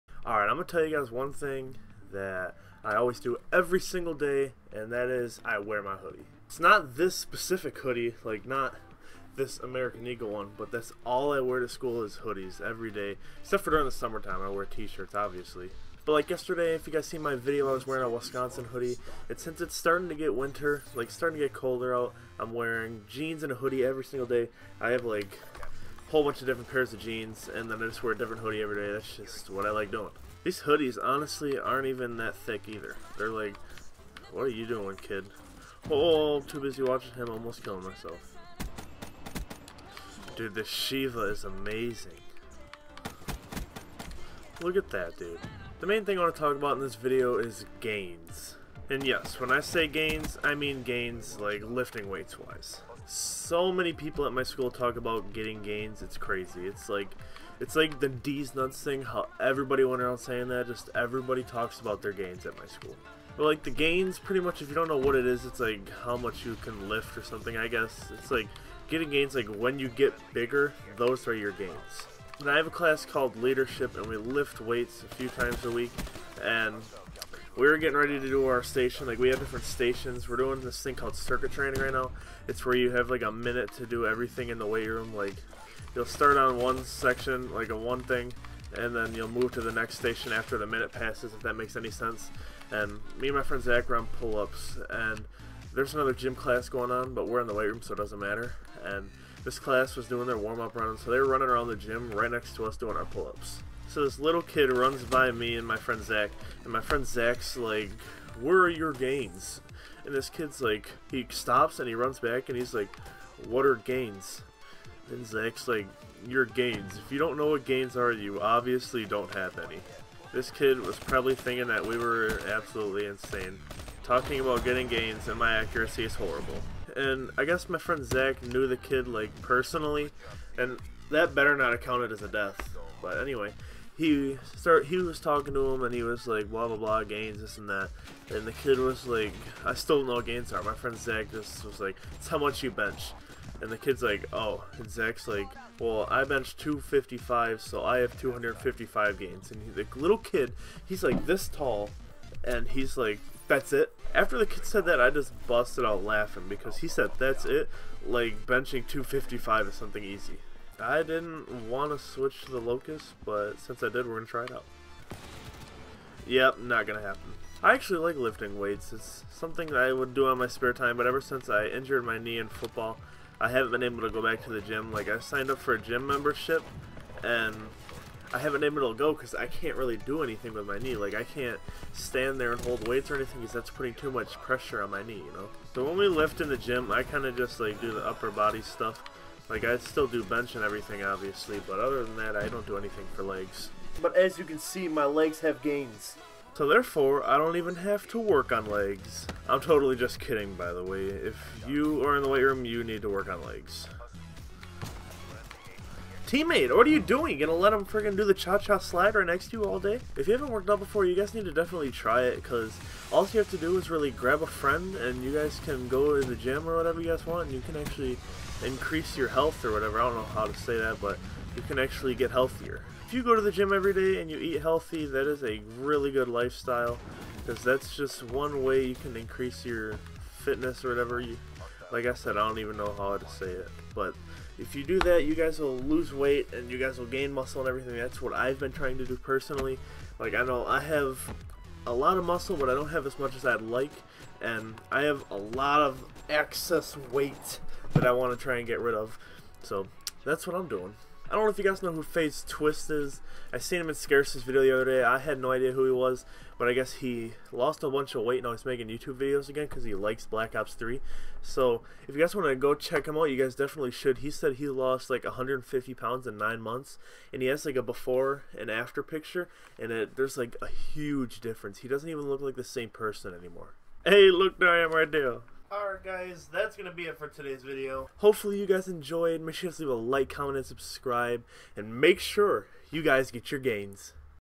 Gains
Hey guys this is a video about getting your gains. Well being in the weight room. its a good thing to eat healthy and workout. anyway i hope you guys enjoy, please leave a like comment and subscribe and ill see you guys in tomorrows video!!!